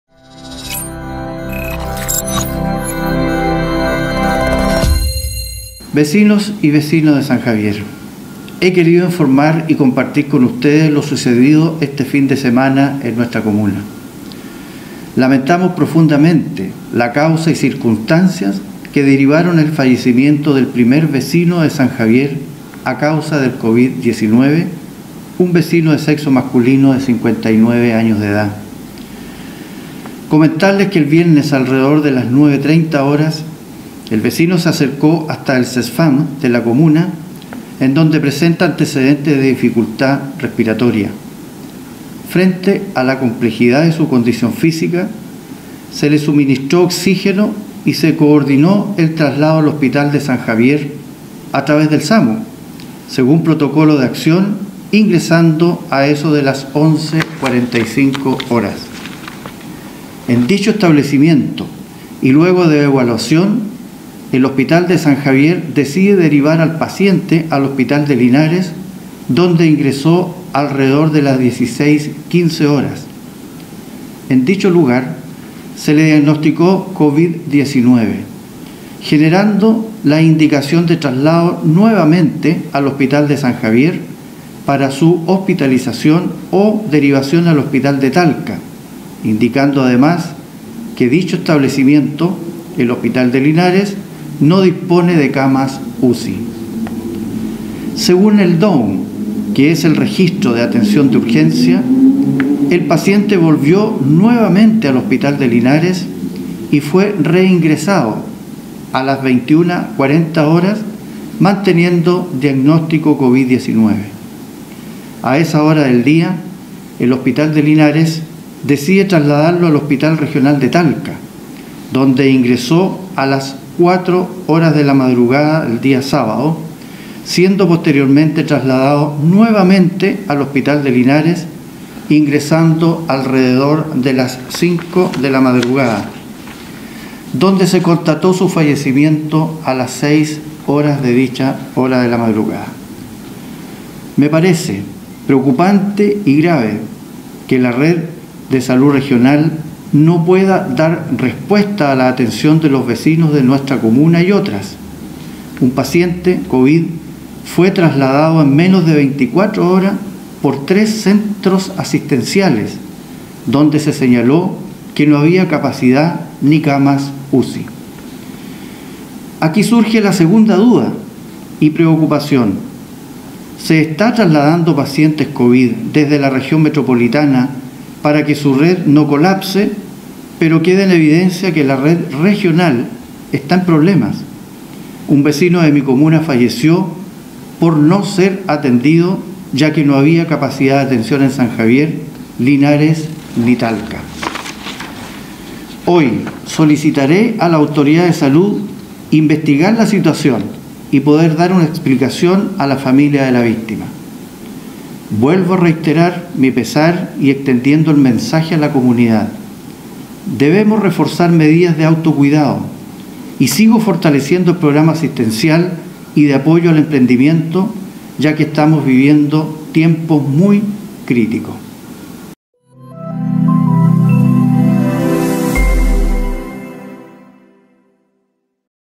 ALCALDE JORGE SILVA, ANUNCIA QUE SOLICITARÁ INVESTIGAR MUERTE DEL PRIMER SANJAVIERINO POR COVID-19.
AHORA ALCALDE JORGE SILVA, ANUNCIA QUE SOLICITARÁ INVESTIGAR MUERTE DEL PRIMER SANJAVIERINO POR COVID-19. LO ANTERIOR POR SITUACION VIVIDA EN EL TRASLADO DEL PASIENTE A DISTINTOS CENTROS ASISTENCIALES, SIN QUE HUBIERA LA POSIBILIDAD DE HOSPITALIZACIÓN.
"EN MENOS DE 24 HORAS FUE TRASLADADO POR 3 CENTROS ASISTENCIALES"